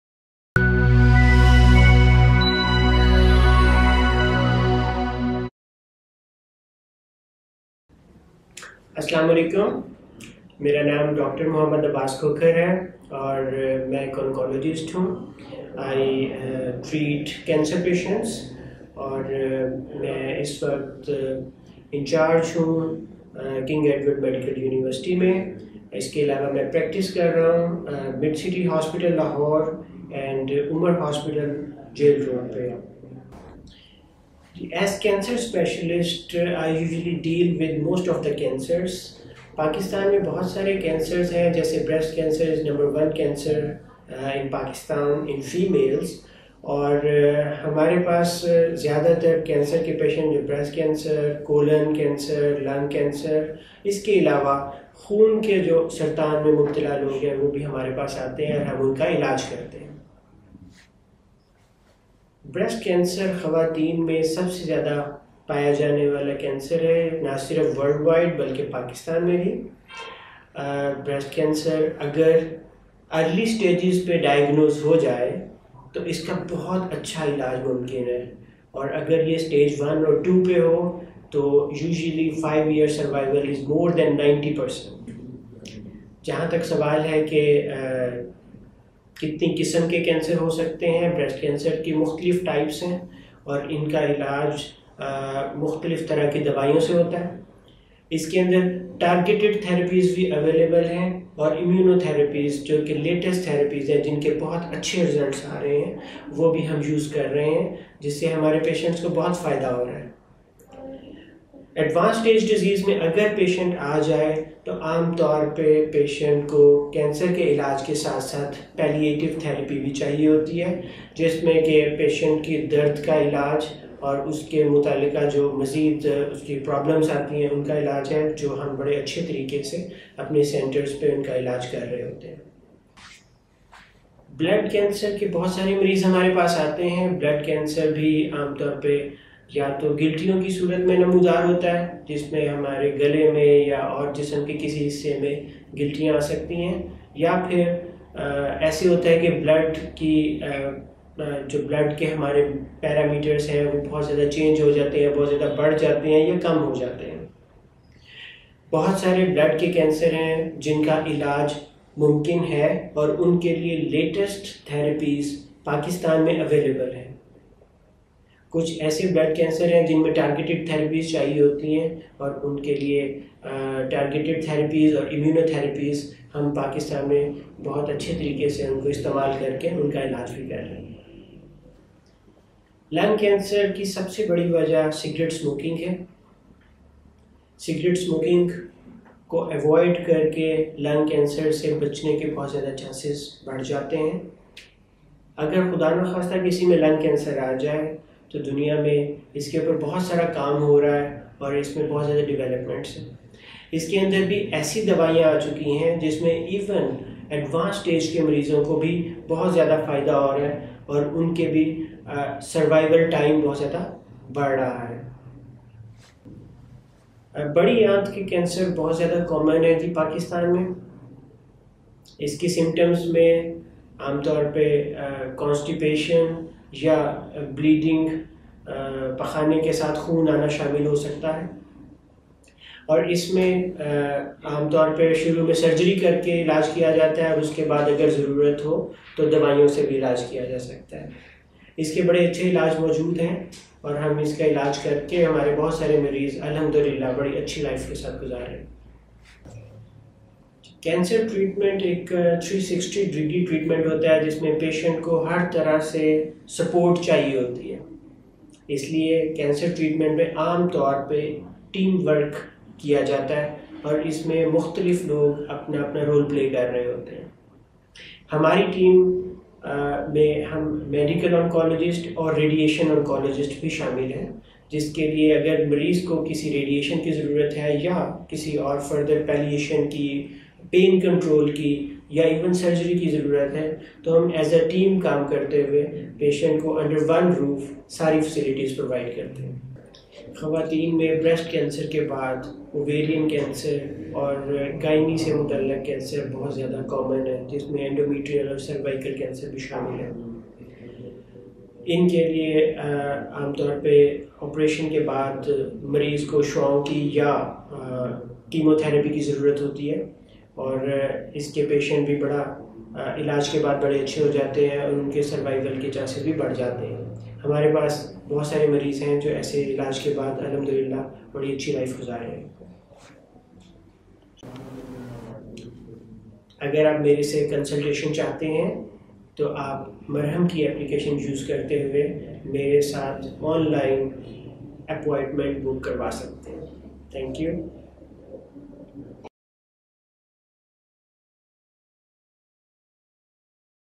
Top Cancer Specialist in Lahore - Dr Abbas Khokhar explaining different types of Cancer in the world
Dr Abbas Khokhar is one of the Top Cancer Specialist in Lahore, as he has 12 years of experience. He has done MBBS, FCPS, MD (USA), DHHM. His Special area of interest is General Medical Oncology.